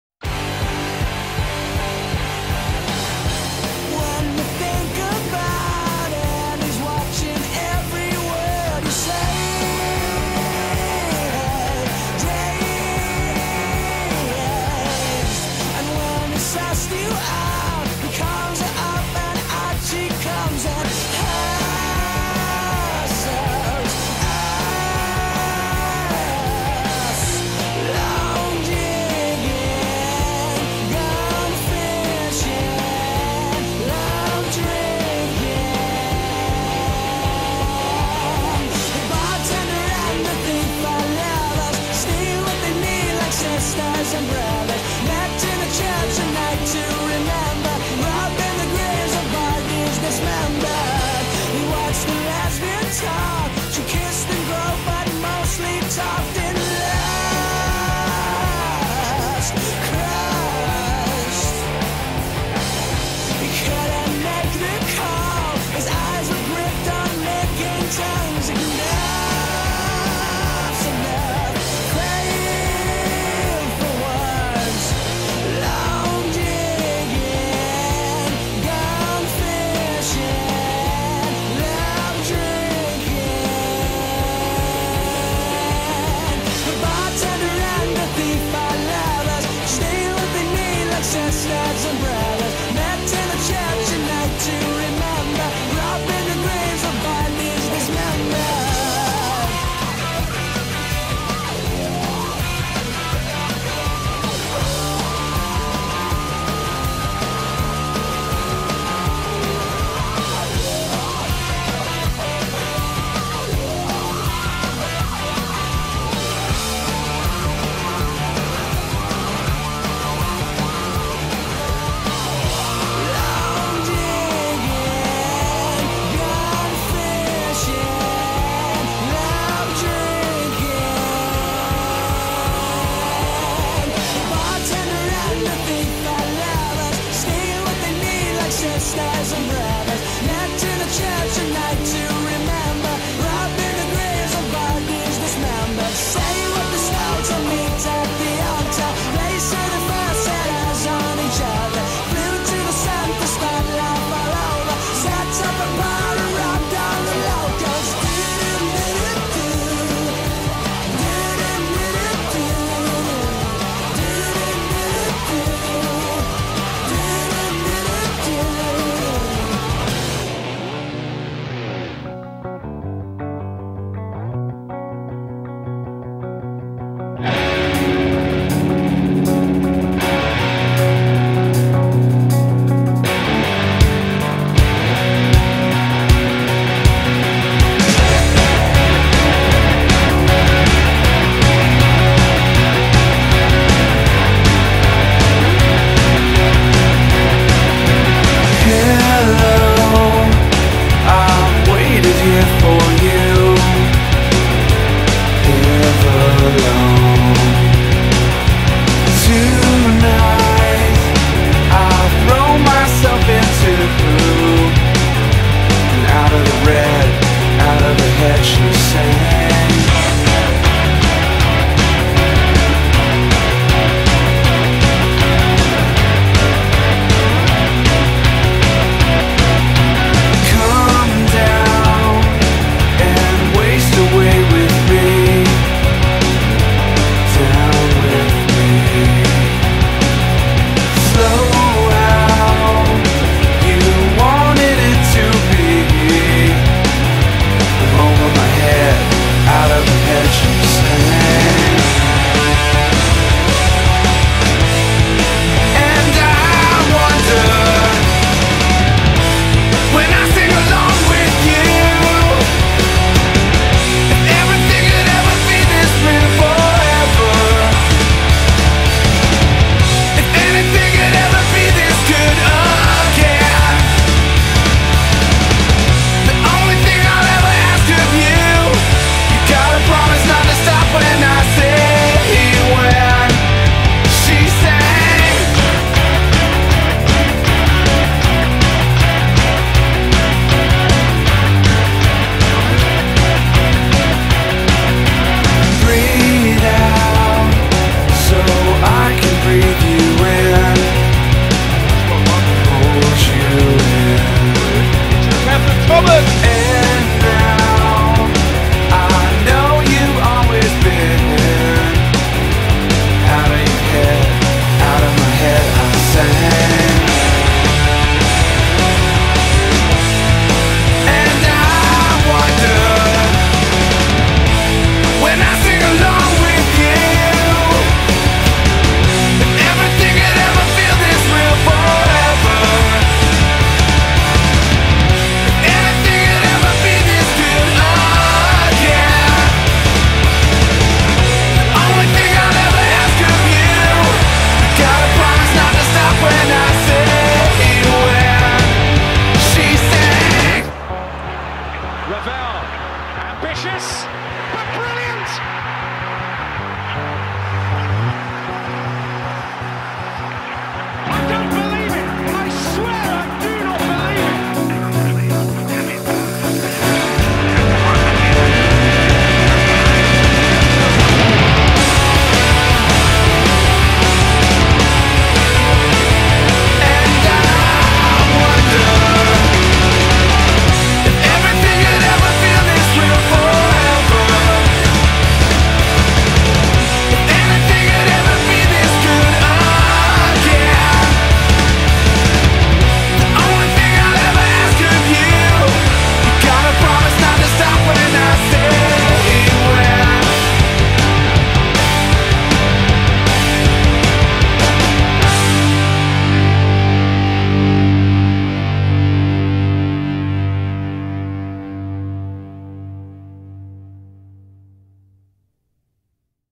Rotherham United Goals 2013/14 - February to May
The final instalment of Rotherham Uniteds goals from the 2013/14 League One season, including the Play-Offs victory to gain promotion to the Championship.

Up The Millers